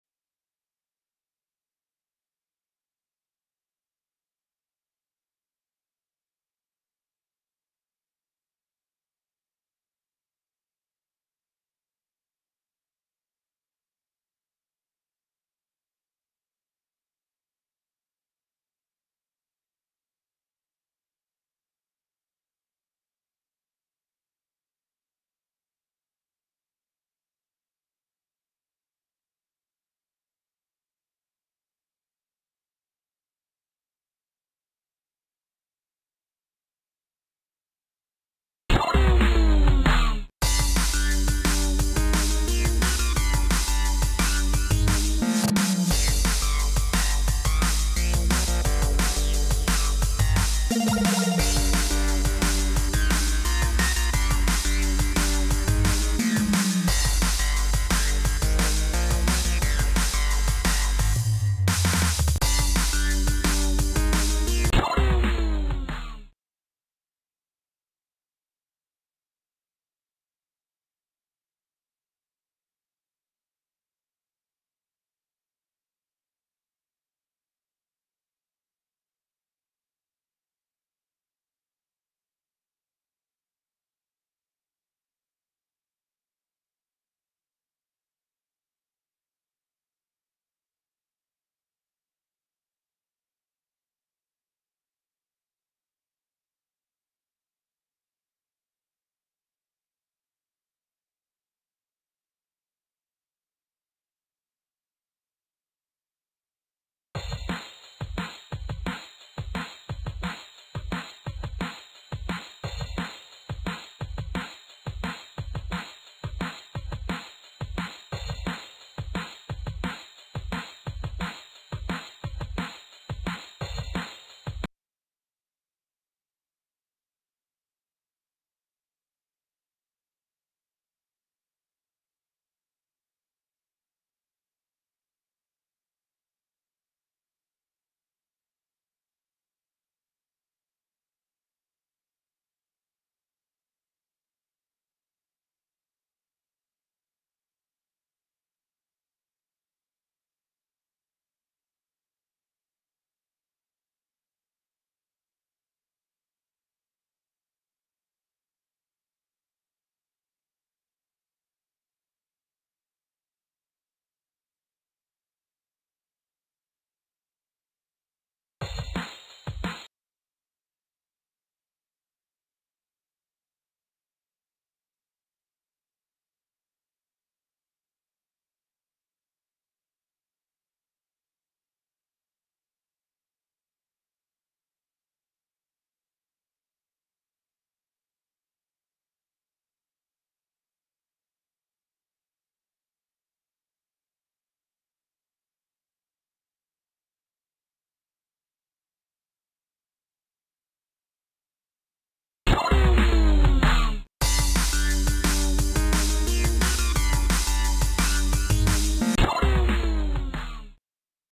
Smash up your clock! The interactive revolution begins...
That old, basic alarm clock is less than you deserve... Wake up! It's time to ditch it and bust out the Kitsound Boom Clock!

* To watch the full interactive version and find out where to buy the Kitsound Boom Clock, please